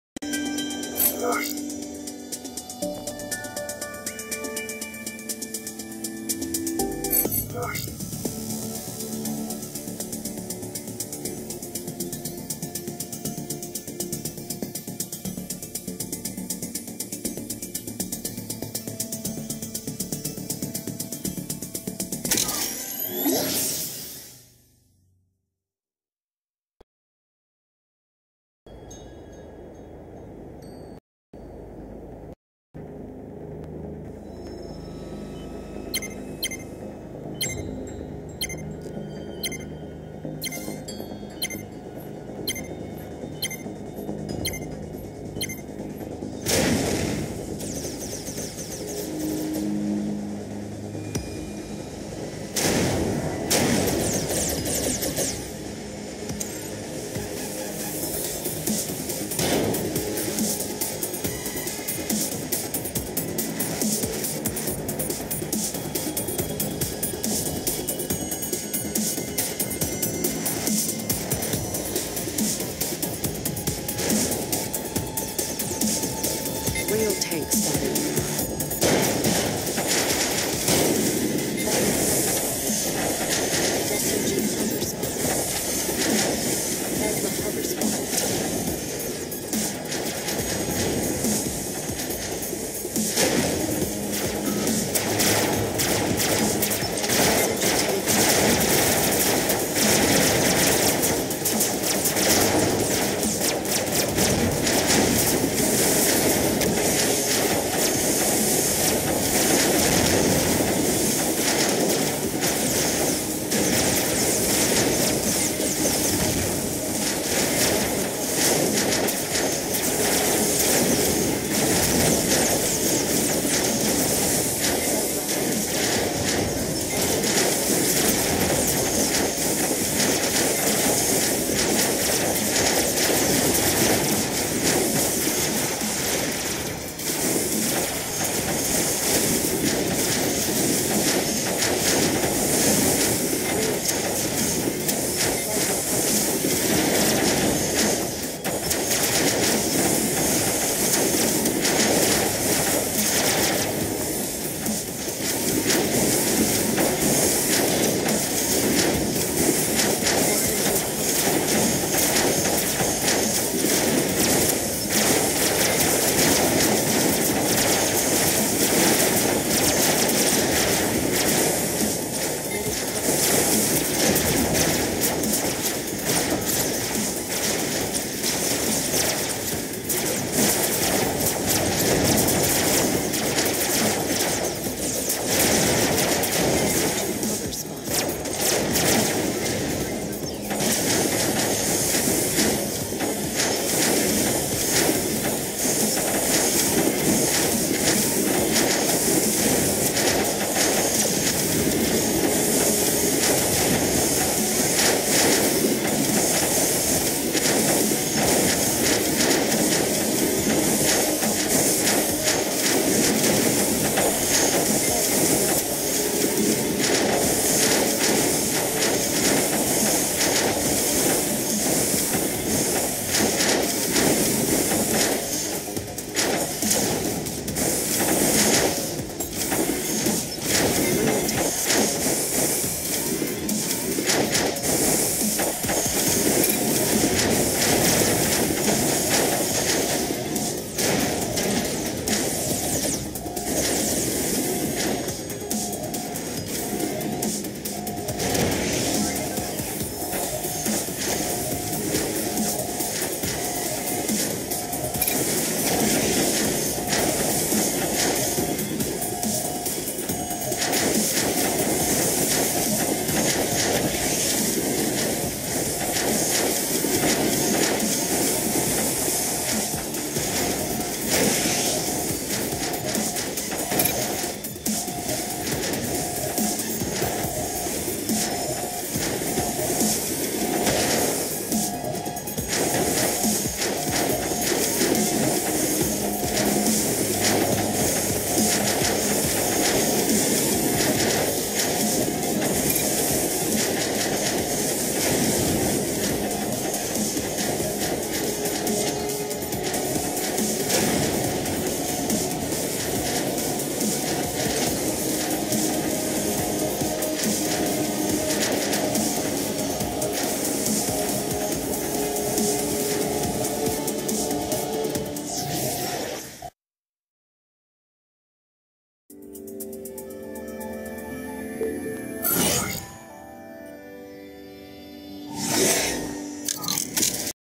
M78D Charon Medium Tank Video E: The Hairiest Lightshow Ever (Ever!)
The M78D Charon Medium Tank gets caught into the most dazzling lightshow ever and against all odds (getting fired upon by multiple enemies on several occasions, getting blocked behind, getting blinded by nanobeams and laser and plasma, etc) survived...